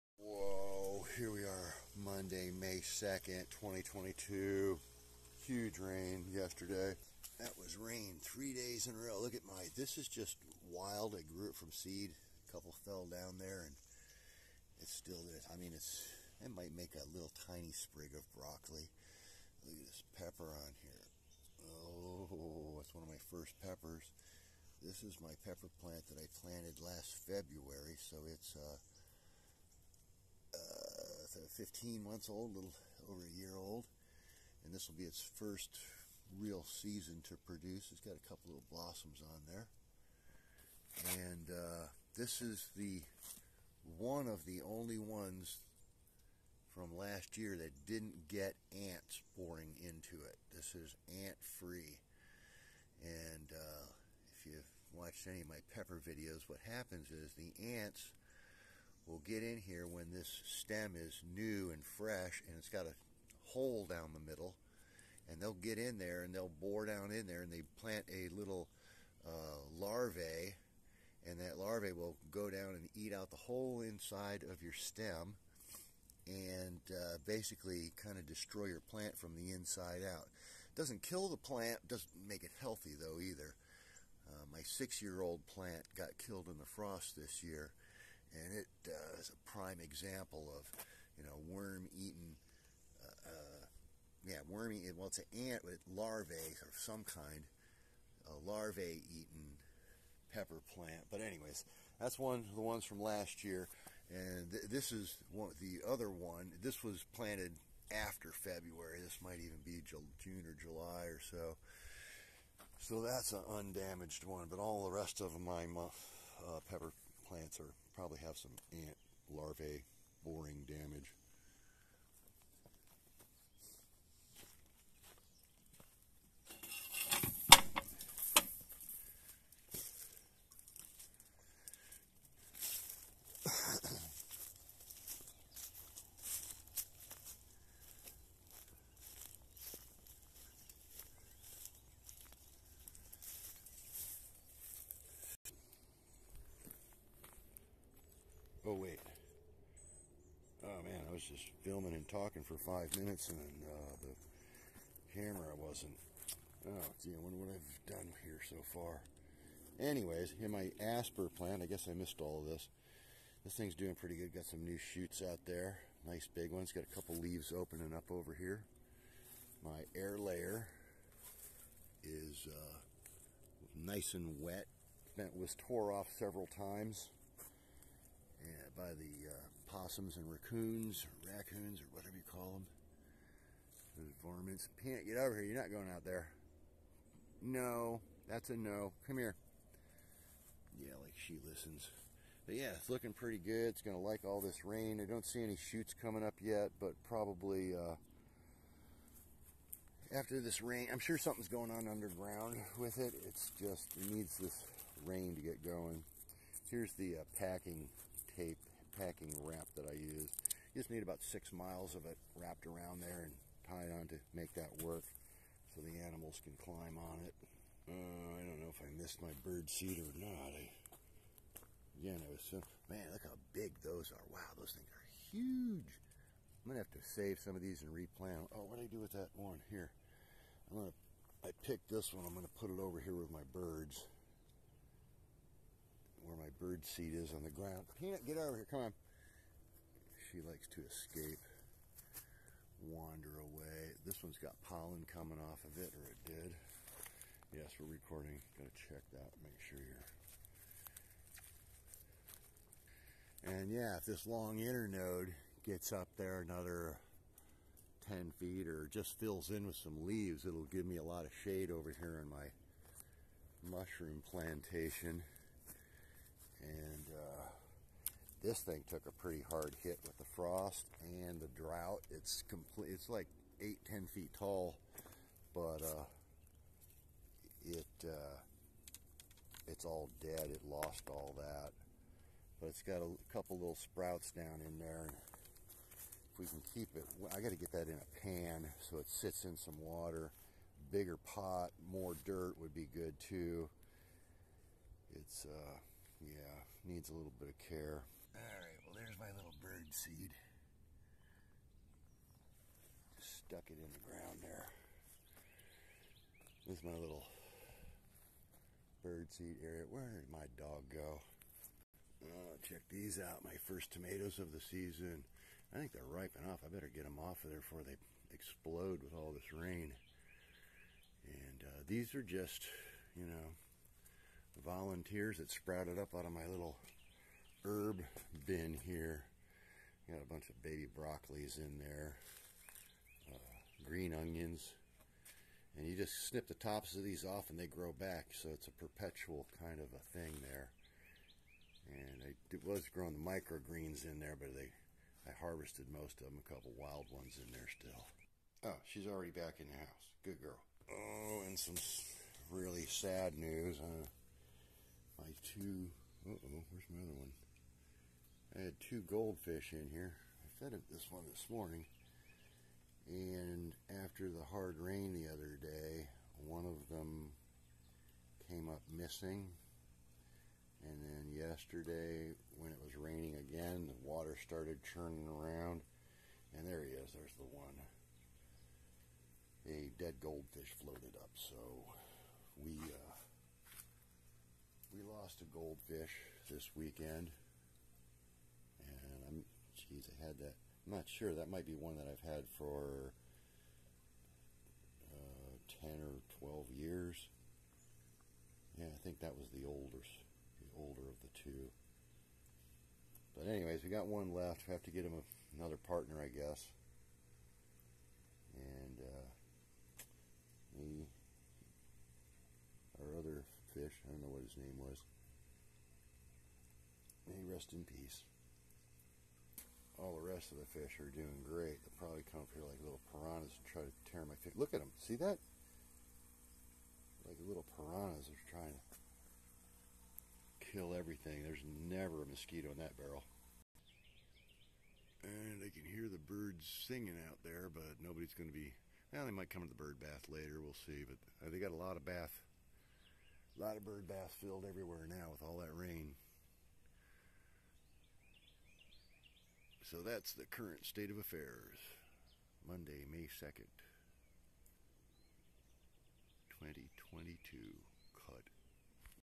Monday May 02 bamboo and birdseed update
we had some more rain for almost 3" in two days after zero rain for two months.